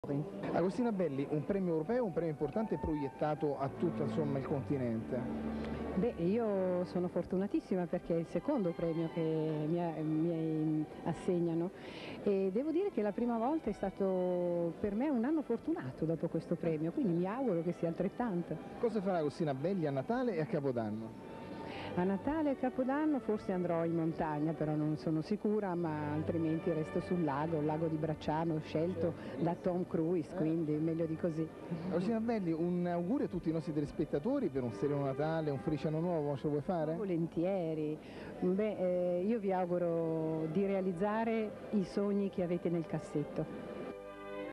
Agostina Belli, omaggio a una grande ed incantevole attrice italiana; intervista di Emanuele Carioti
AgostinaBelli, al Premio Personalità Europea 2006 mio piccolo omaggio alla grande attrice italiana, Protomoteca del Campidoglio, Roma, mercoledì 13 dicembre 2006, EmanueleCarioti T9.
Emanuele Carioti - Giornalista, youtuber & Blogger